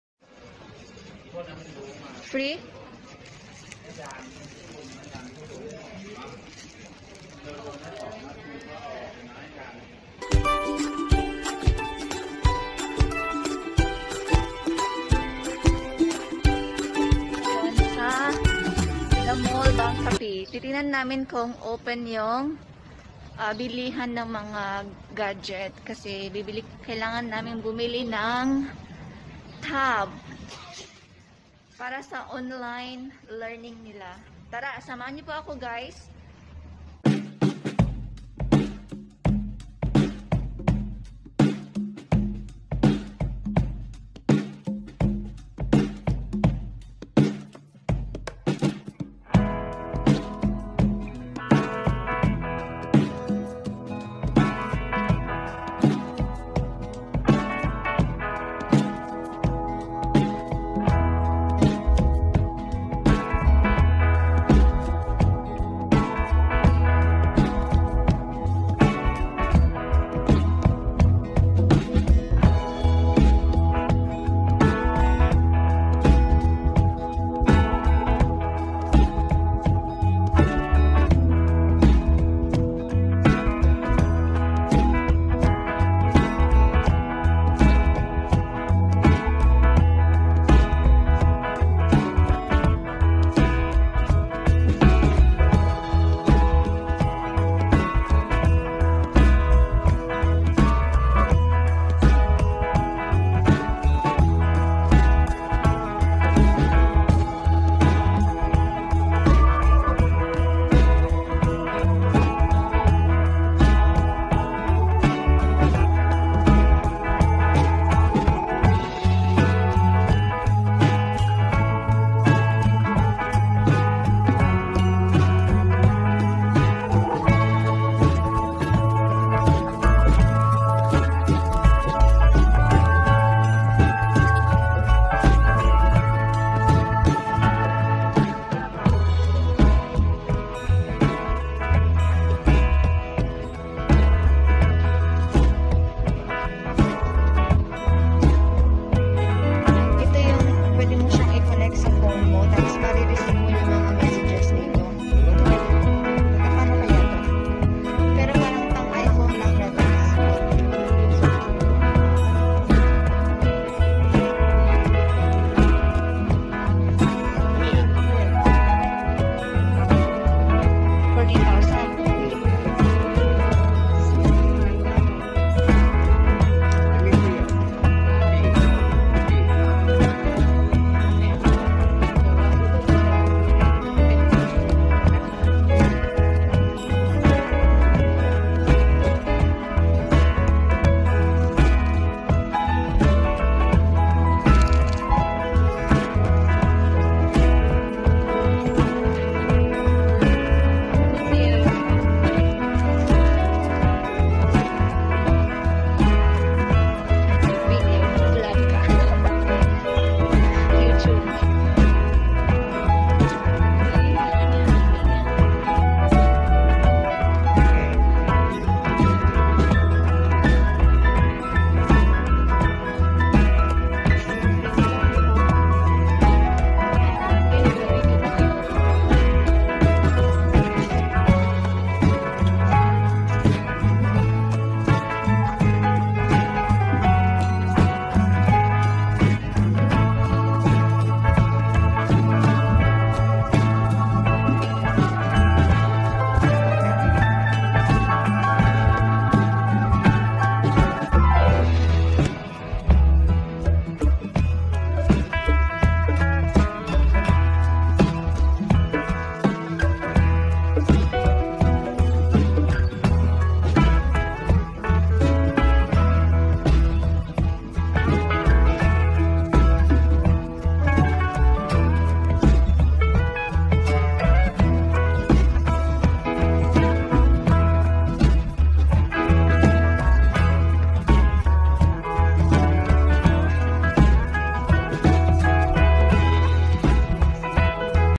SAMSUNG GALAXY TAB S6 LITE swak sa budget
A writing, drawing online learning tablet for my kid.